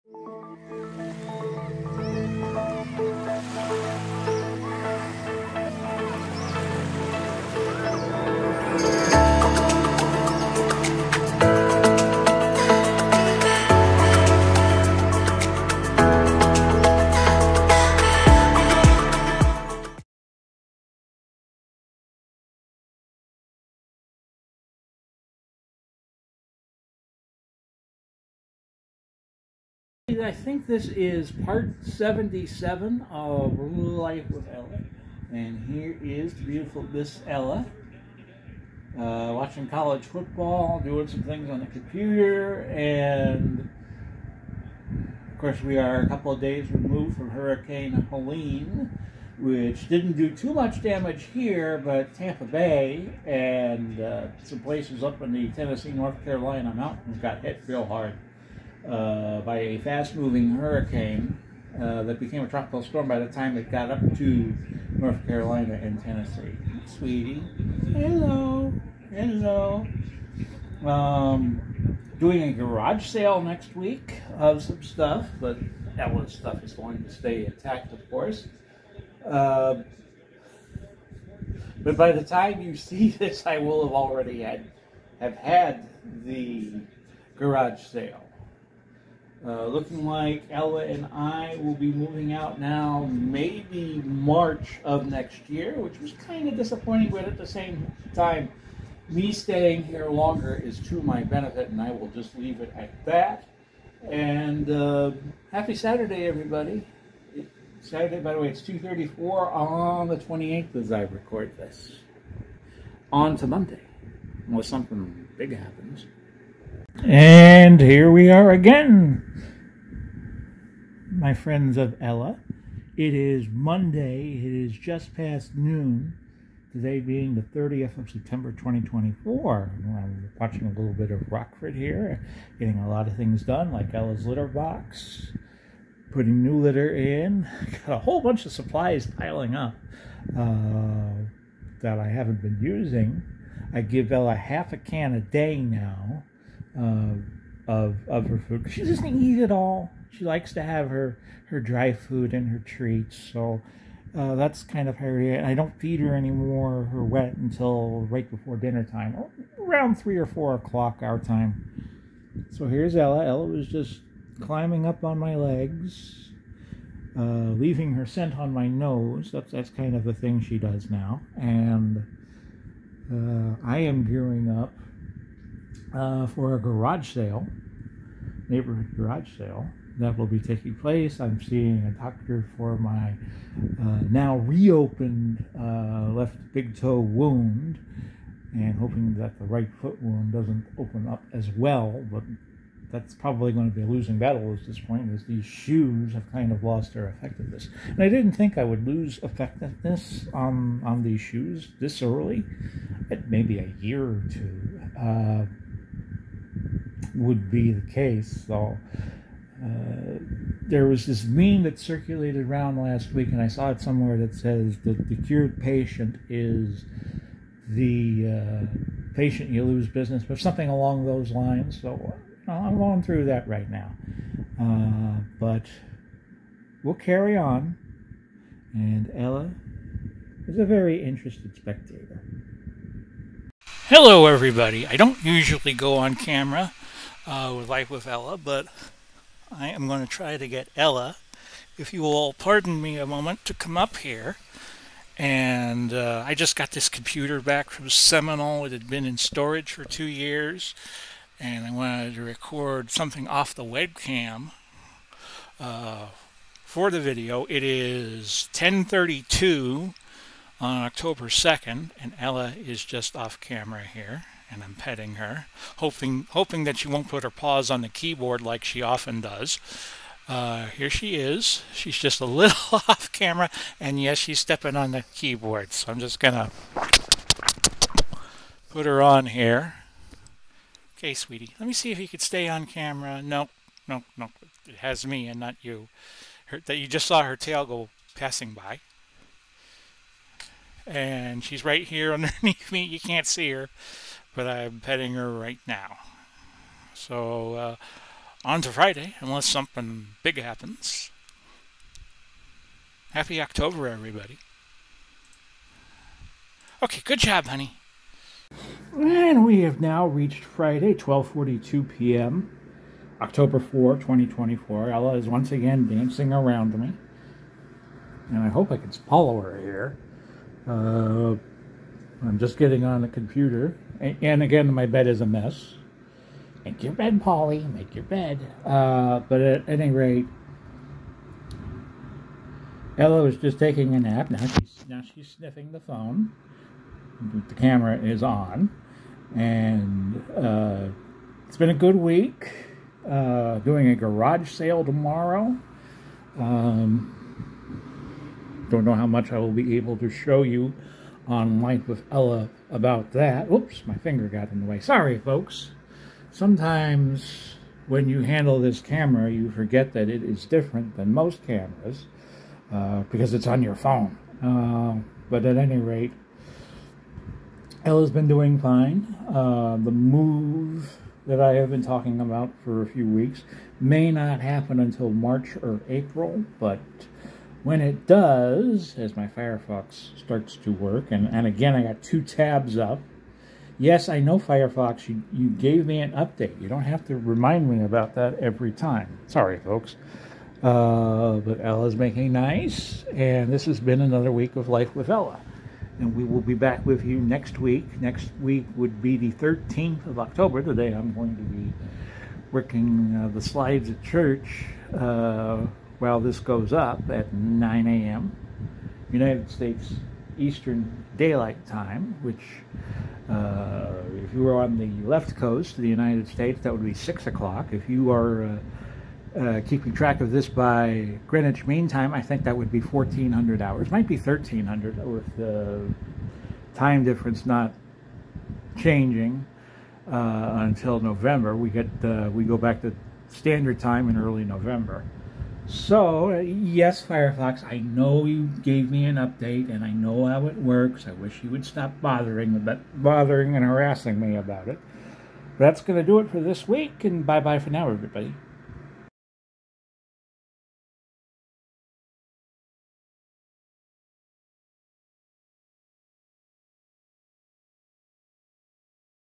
Life With Ella, Part 77: Hello October
Another new month starts, and Ella and I talk about what's new, kind of.